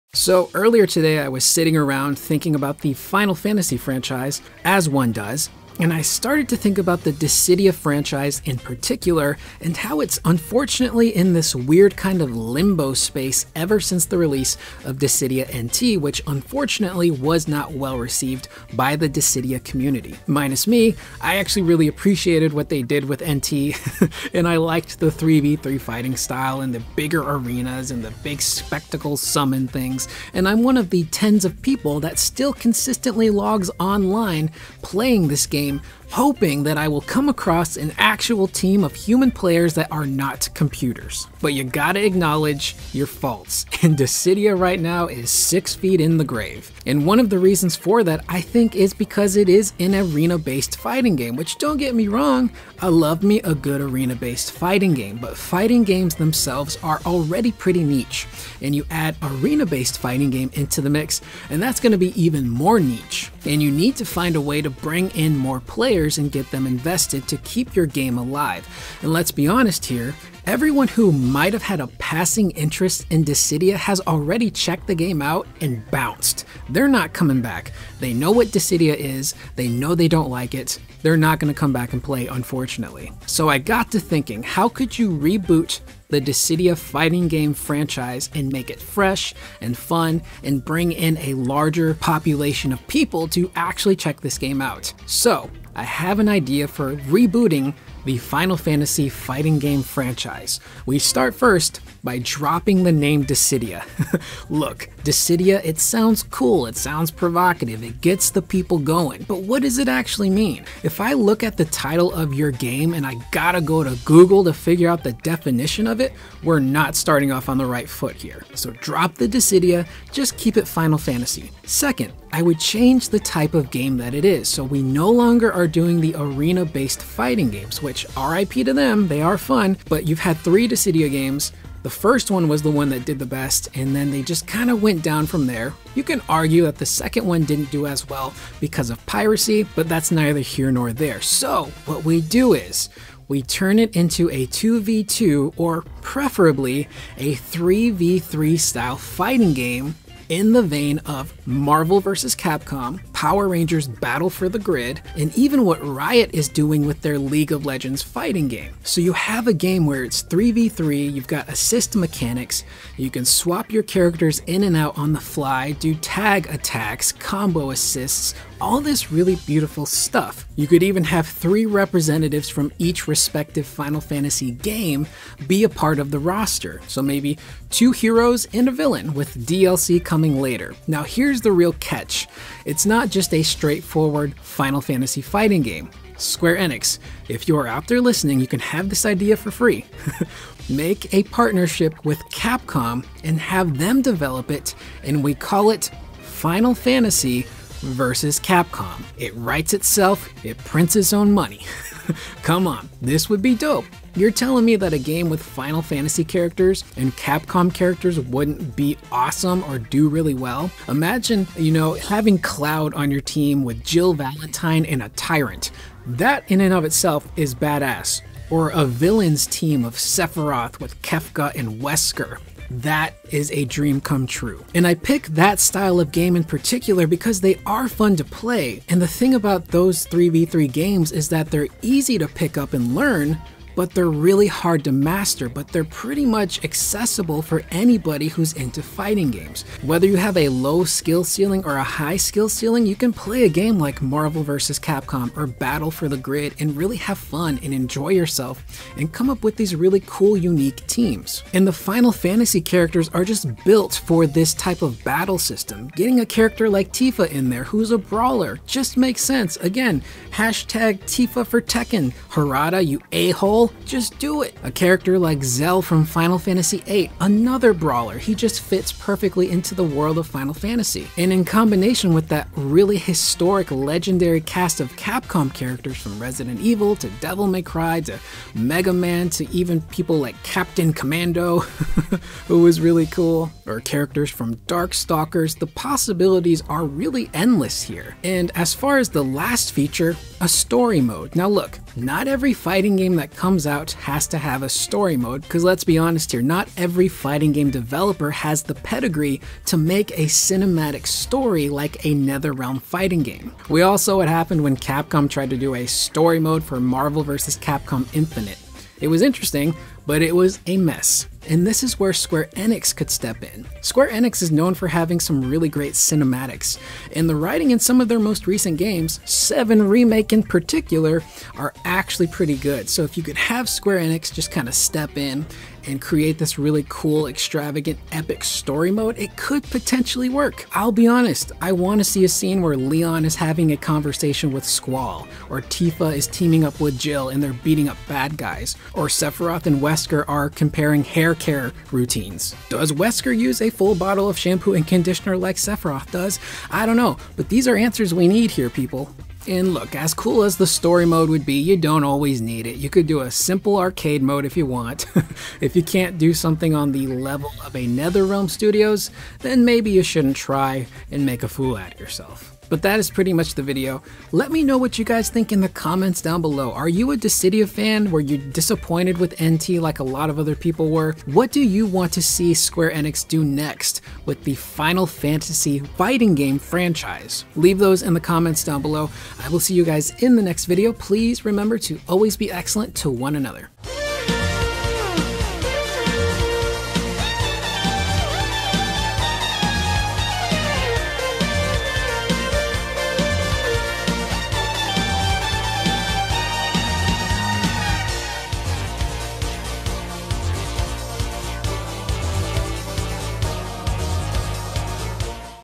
How I'd Reboot Dissidia into My Dream FINAL FANTASY Fighting Game | Rebooting Dissidia Series
Join me as I take the DISSIDIA franchise on a rebooted journey to transform it into a 3 V 3 tag based fighting game.